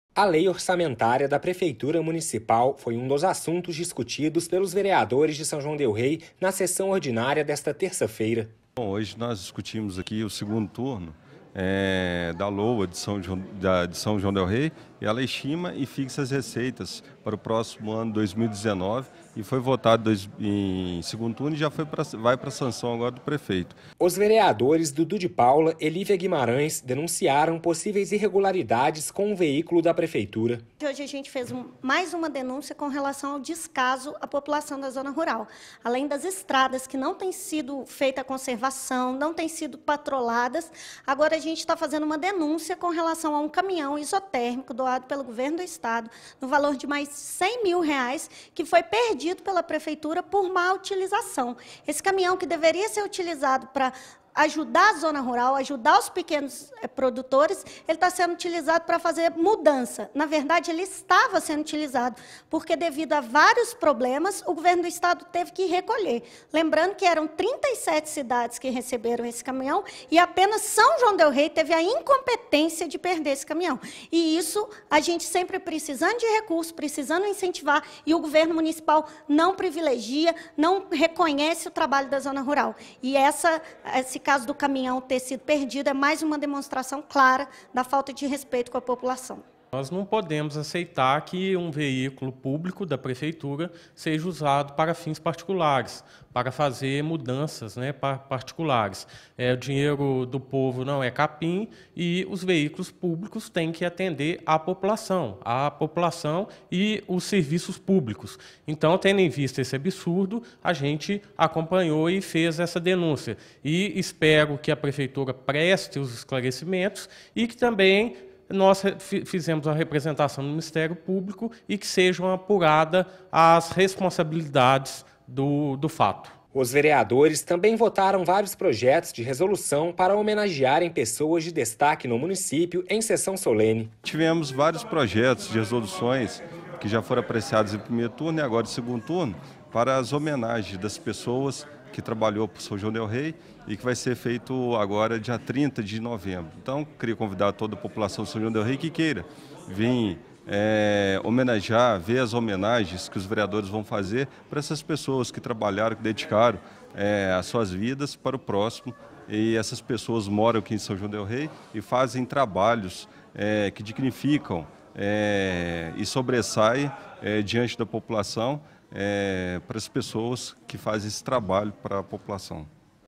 Vereadores se reúnem em sessão ordinária
Os vereadores de São João del-Rei se reuniram em sessão ordinária na tarde da última terça-feira, dia 20 de novembro.